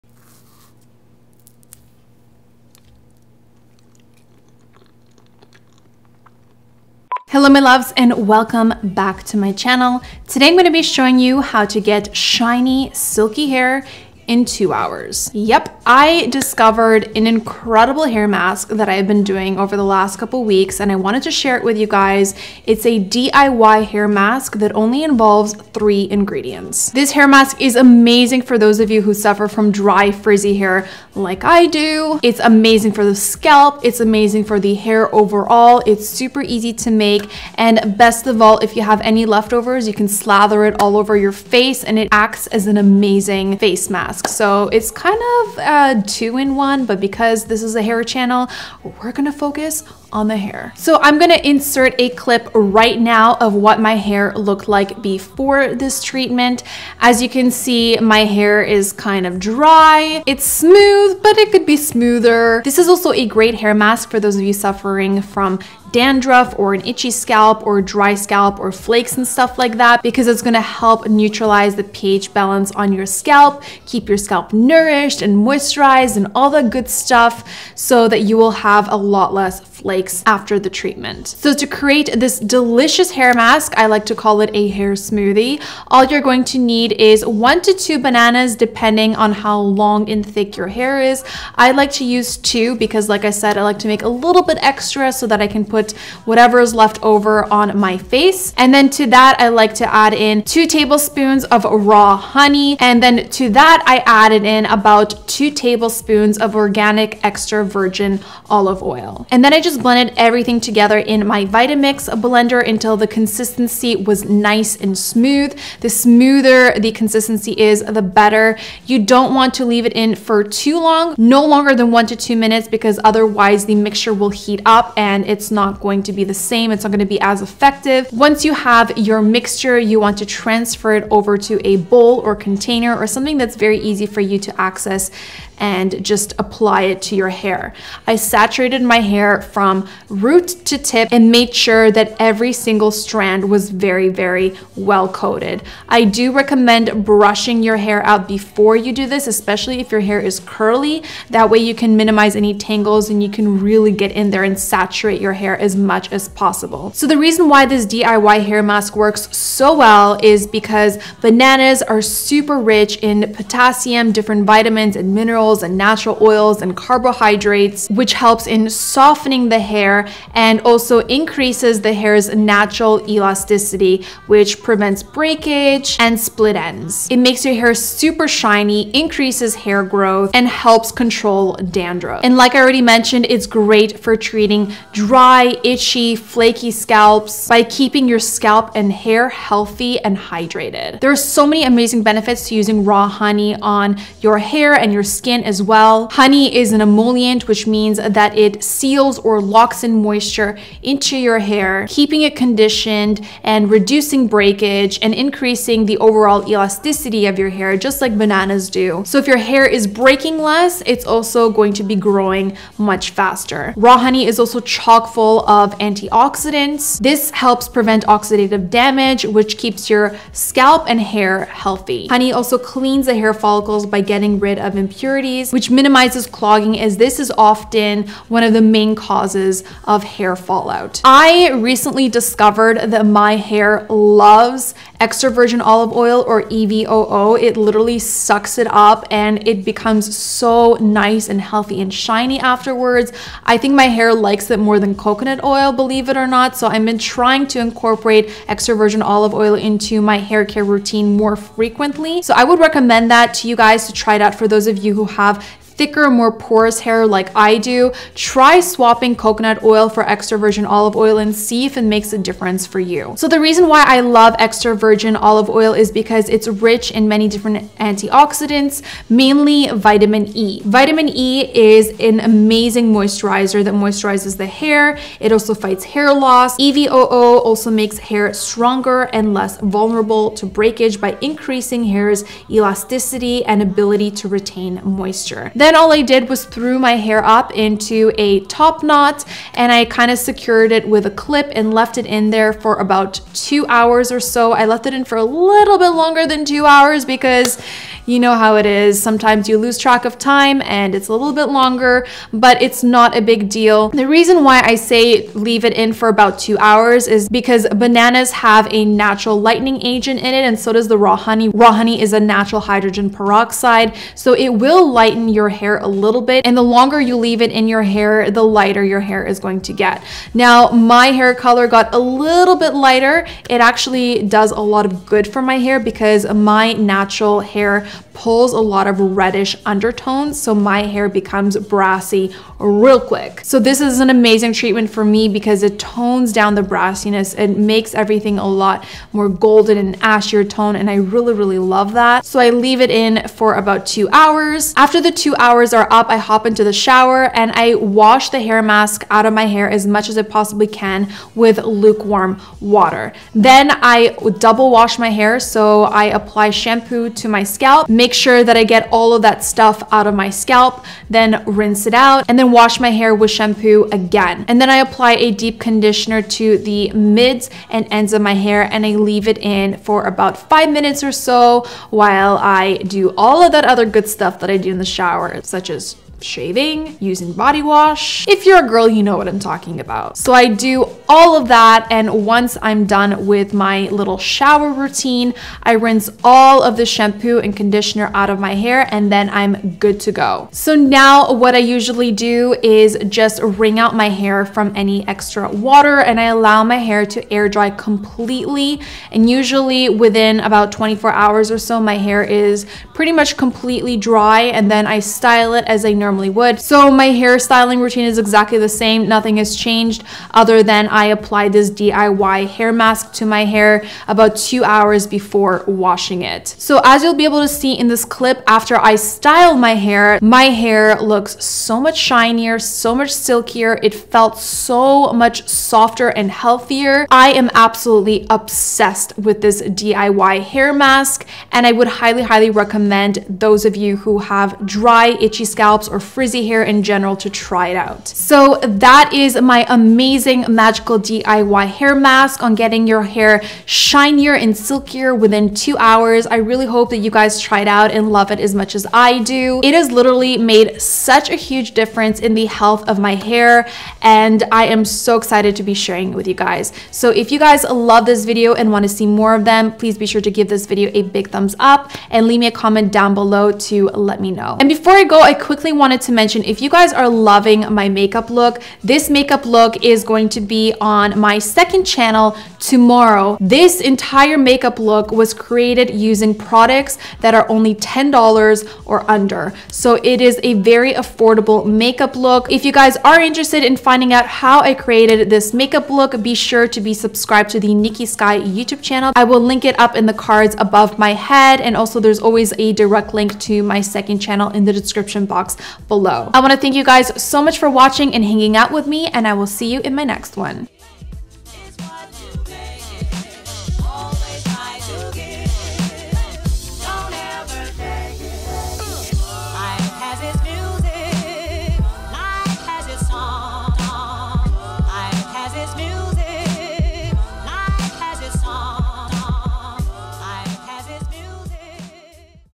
Silky Smooth Hair in 2 Hours | DIY Hair Mask
This DIY hair mask is a game changer and will give you shiny, silky hair in 2 hours! I'm completely obsessed and it has made such a huge difference in the health of my hair lately! Hope you try it out and share your results with me :) Xx

OUTFIT DETAILS

FOLLOW FANCY HAIR

WRITE TO ME HERE:

Niki Sky
8171 Yonge Street, Suite 365
Thornhill, ON
L3T 2C6
Canada